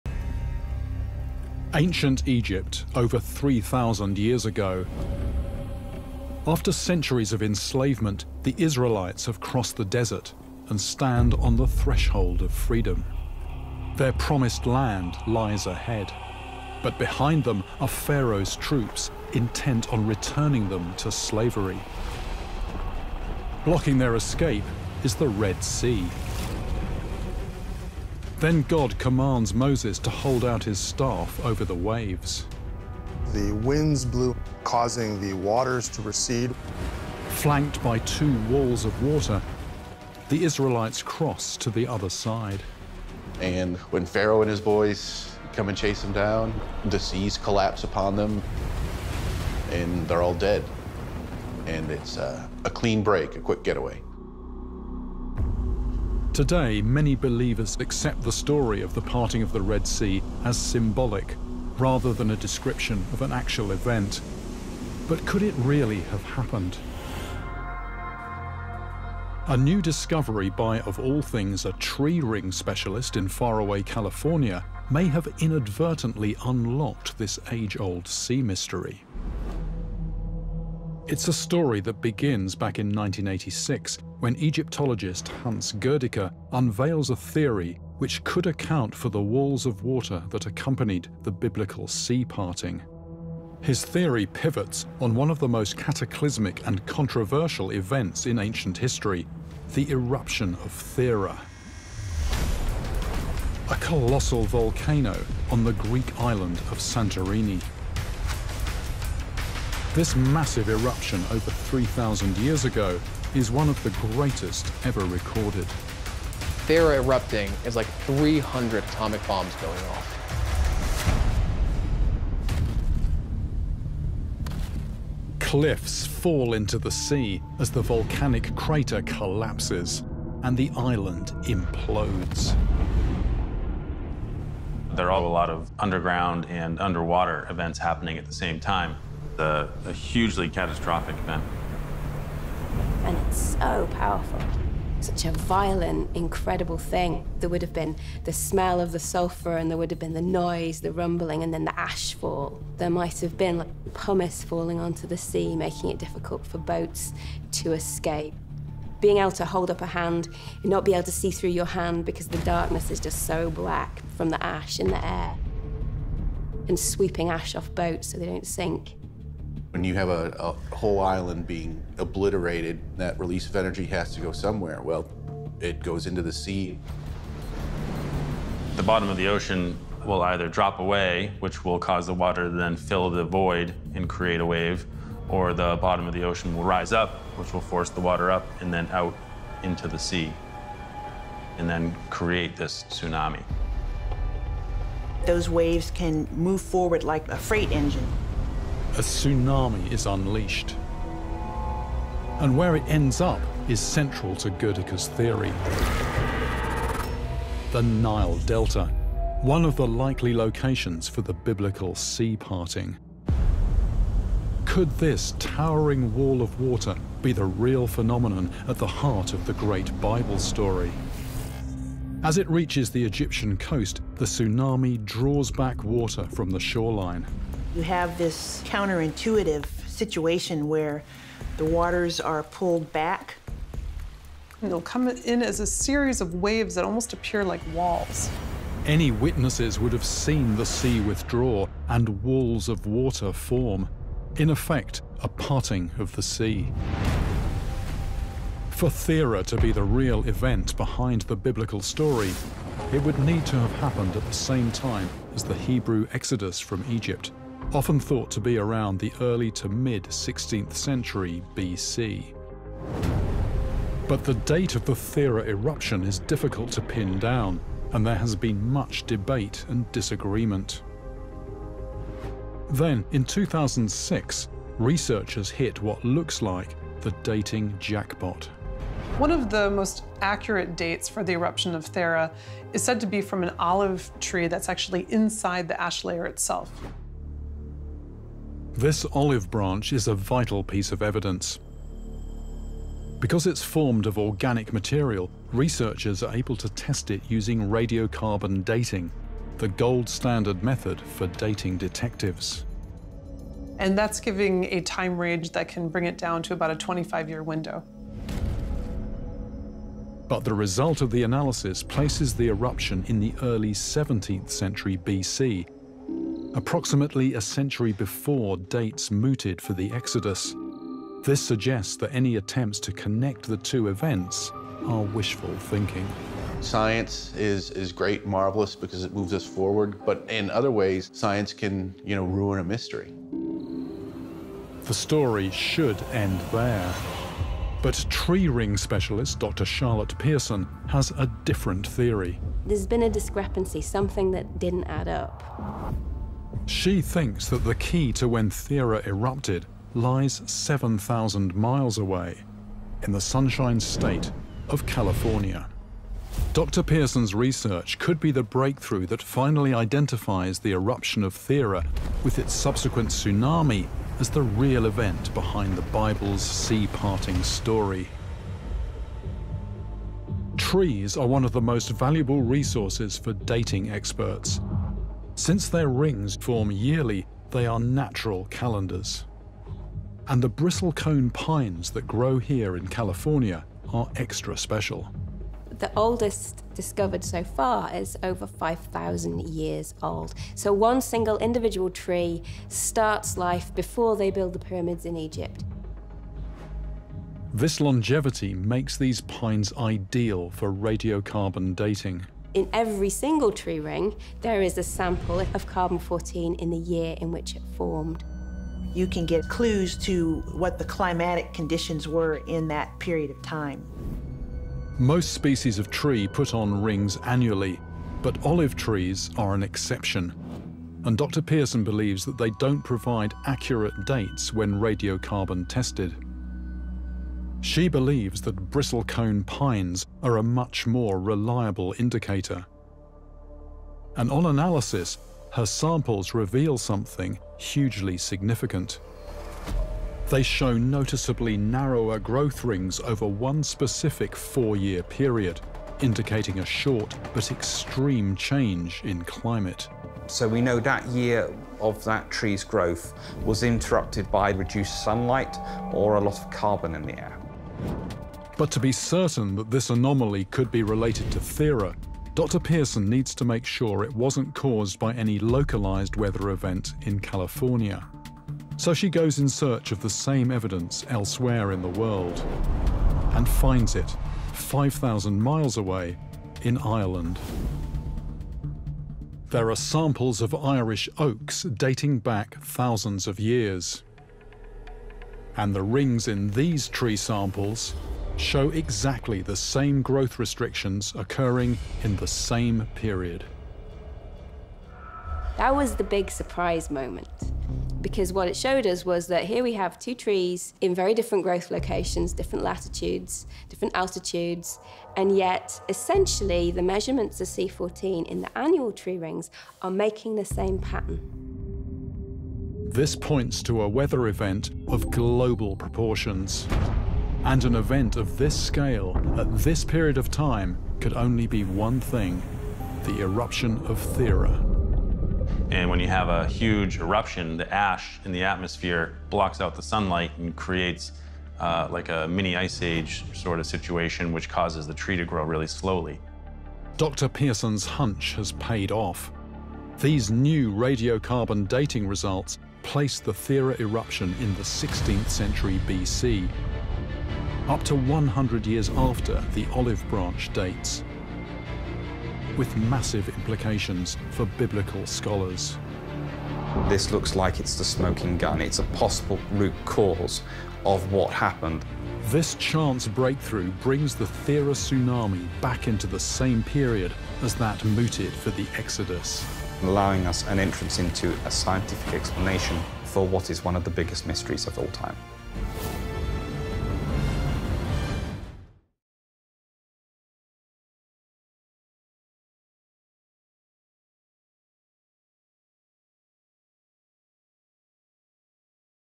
Could The Biblical Parting Of The Red Sea Actually Have Happened? | Mysteries Of The Deep
Many people believe that when Moses parted the Red Sea, it was more metaphorical and symbolic than literal… but could it have actually happened? Scientists believe they may have discovered how such an extraordinary event could have been possible.